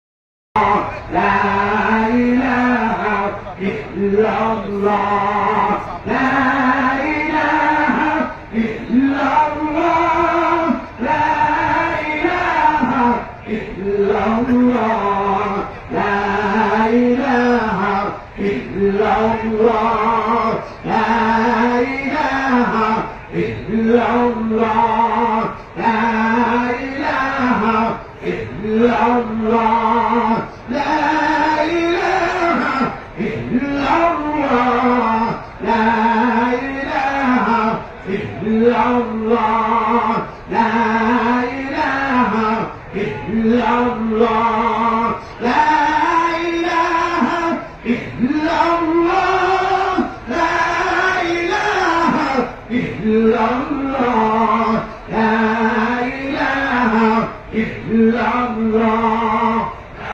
পবিত্র রমজানে কিছু সময় মাওলা পাকের জিকির করি। কলব পরিষ্কারের সবচেয়ে বড় হাতিয়ার মাওলা পাকের জিকির
আসুন পবিত্র রমজানে কিছু সময় মাওলা পাকের জিকির করি। কলব পরিষ্কারের সবচেয়ে বড় হাতিয়ার মাওনা পাকের জিকির।
চরমোনাই ময়দানে আল্লাহর পাগলদের সাথে জিকিরের স্বাদ অন্যরকম।